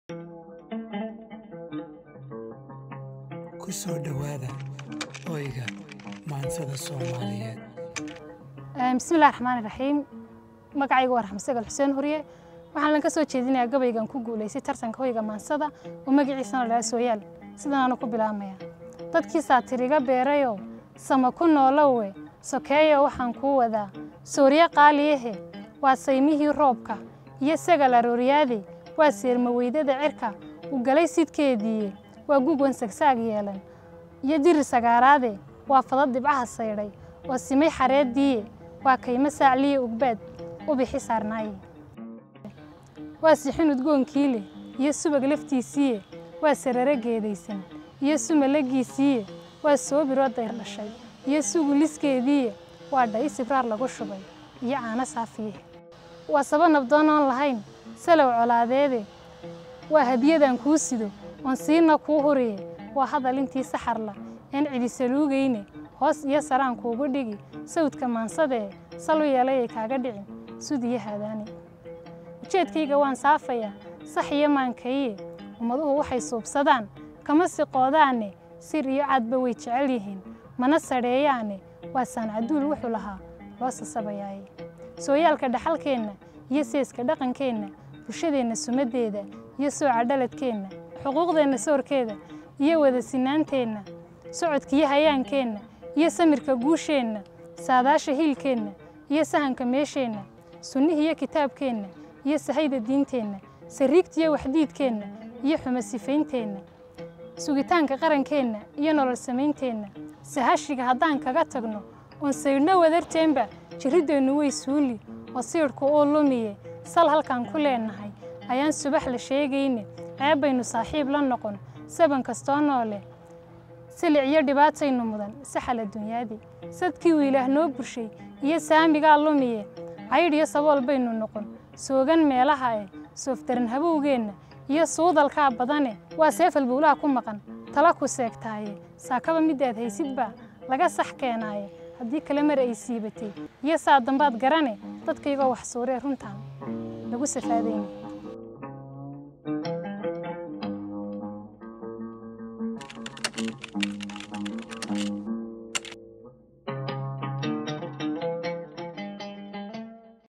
Raxma Huseen Huriye: 2021 Home of Somali Poetry Woman Poet of the Year.
Raxma Huseen Huriye performs her poem, Soyaal, which won the 2021 Home of Somali Poetry Woman Poet of the year award.